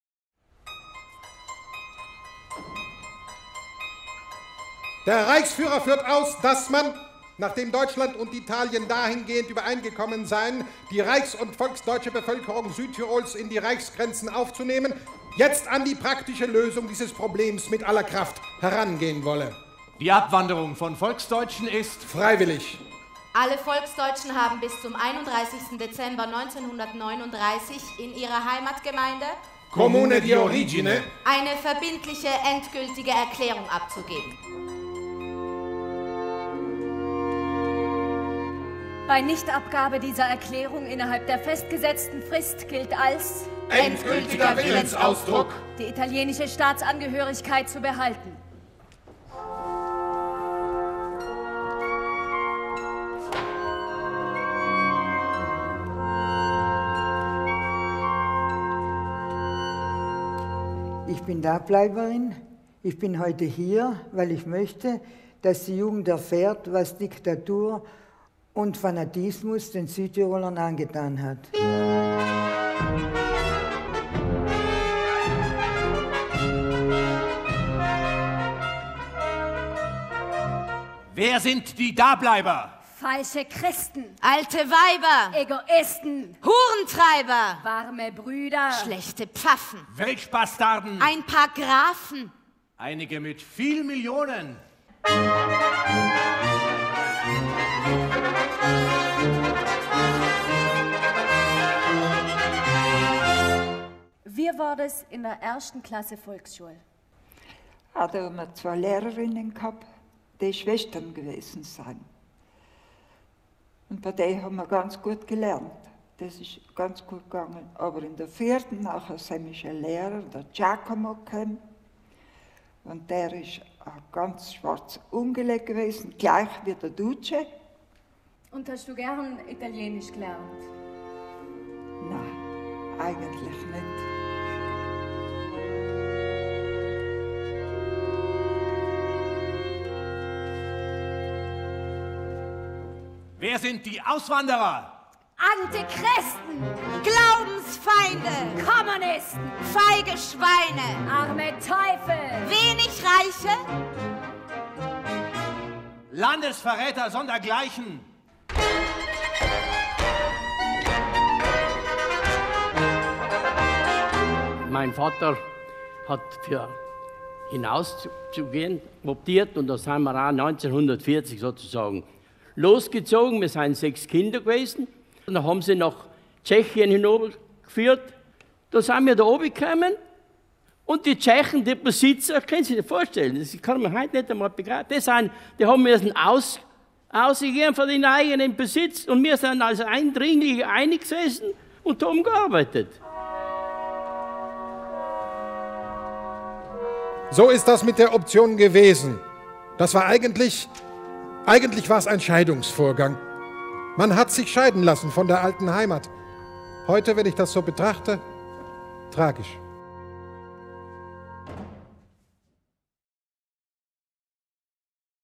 Option. Letzte Spuren der Erinnerung. 2019 - TRAILER
Option. Letzte Spuren der Erinnerung

Gehen oder bleiben – 1939 ging ein Riss durch die deutschsprachige Südtiroler Bevölkerung. Sie wurde vor die Wahl gestellt, ins Deutsche Reich auszuwandern oder in Südtirol zubleiben und ihre deutsche Sprache und Kultur  aufzugeben. 80 Jahre danach erinnern sich Zeitzeuginnen und Zeitzeugen an die schwere Entscheidung. Optanten, Dableiber, Rücksiedler und Nichtrückoptanten erzählen live auf der Bühne von Abschied und Heimat, sie brechen das Schweigen. Ihre persönlichen Geschichten stehen im Zentrum des außergewöhnlichen Theaterprojekts „Option. Letzte Spuren der Erinnerung“.

Die Musicbanda Franui aus Osttirol begleitet den Abend mit neu arrangierten Liedern. Ein bewegendes Dokumentartheaterstück mit Live-Musik, das Erinnerung, Authentizität und die Kraf des Erzählens vereint. Freuen Sie sich anlässlich des 80. Jahrestages auf die letzten zwei Vorstellungen der bislang erfolgreichsten Theaterarbeit der Vereinigten Bühnen Bozen.